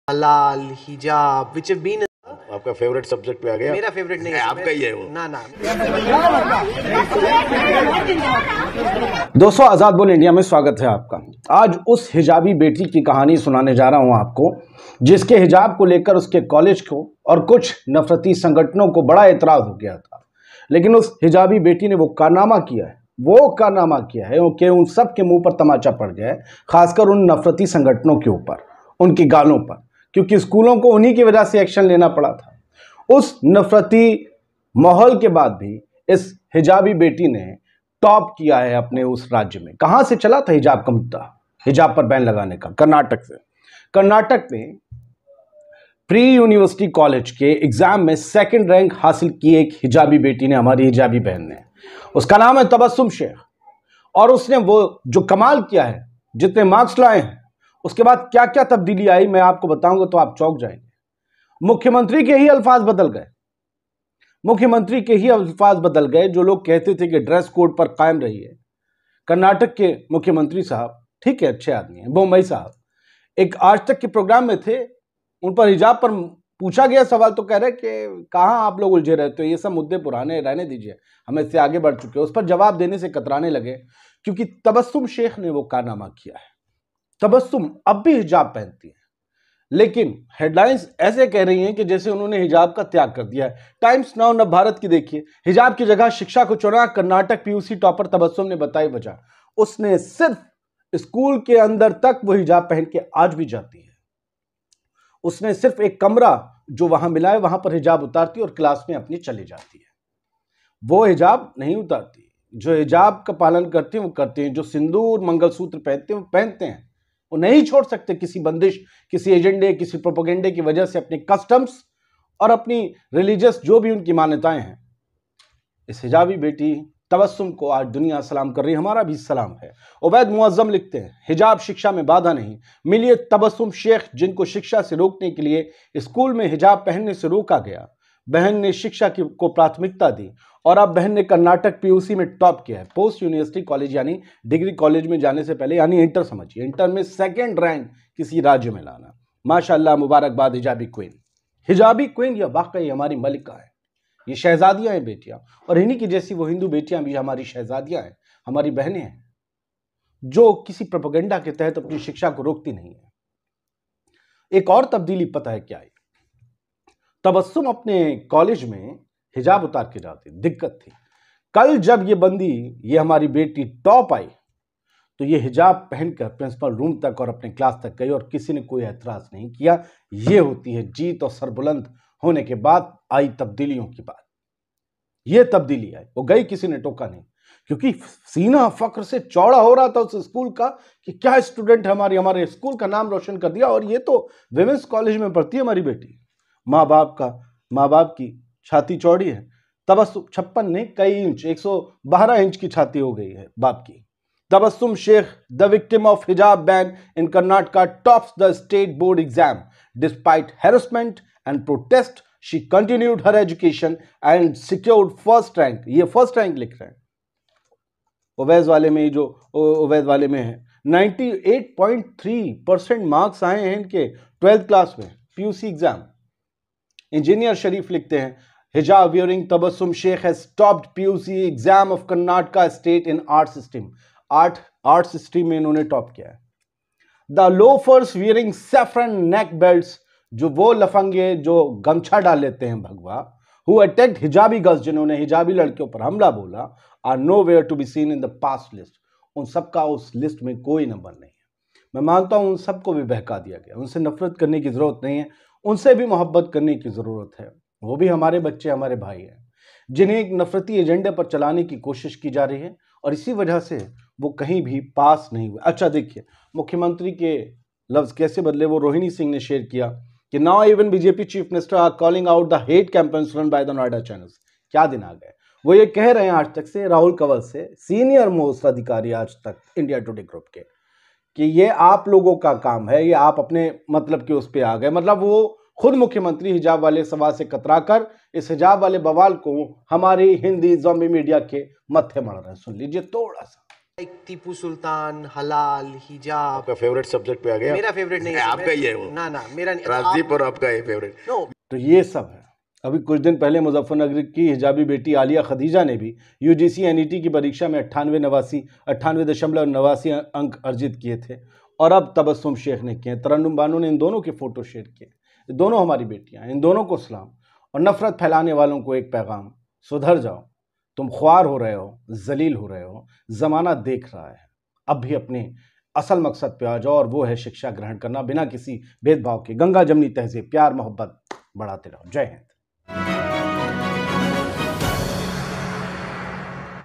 Hijab girl tabassum sheikh बनी karnataka PUC topper | CM Bommai slams rahul kanwal on hijab
Hijab girl tabassum sheikh बनी karnataka PUC topper
CM Bommai slams rahul kanwal on hijab